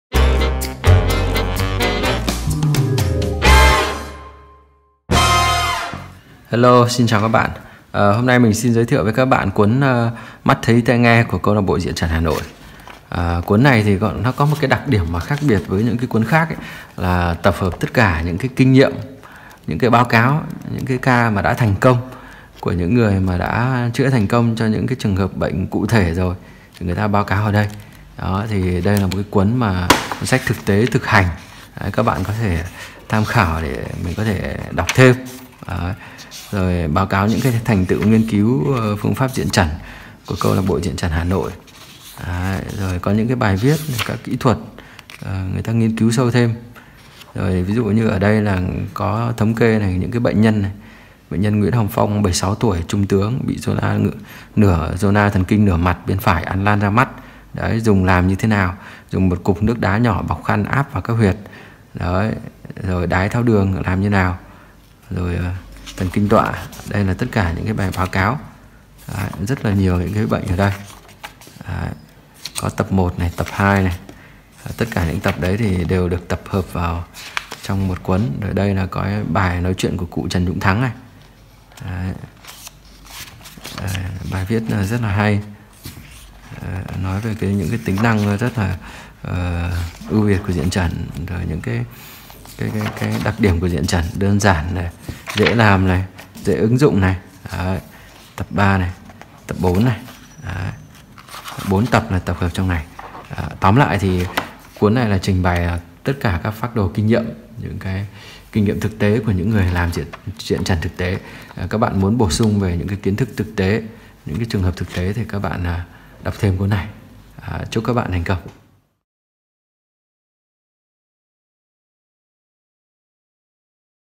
Mắt thấy tai nghe - Kinh nghiệm thực tiễn Diện Chẩn
Sách photo khổ A5 - 145 trang
Nội dung;
Những lời chỉ đẫn của tác giả phương pháp Diện Chẩn - Gs.TSKH Bùi Quốc Châu rất chân tình cởi mở, với kỹ thuật điêu luyện sáng tạo...chữa bệnh ngay thực tế có hiệu quả, gây nhiều ngạc nhiên thật là "mắt thấy tai nghe"

Vì vậy chúng tôi cho rằng đây là món quá quý giá cần phổ biến rộng rãi cho những ai ưa thích chữa bệnh không dùng thuốc theo phương pháp Diện Chẩn Điều Khiển Liệu Pháp.

Trân trọng giới thiệu với các bạn tập tài liệu lấy tựa đề là "Mắt thày tai nghe" để các bạn có thể nghiên cứu sưu tầm, ứng dụng sáng tạo thực hiện việc chăm sóc sức khỏe bản thân, gia đinh và cộng đồng!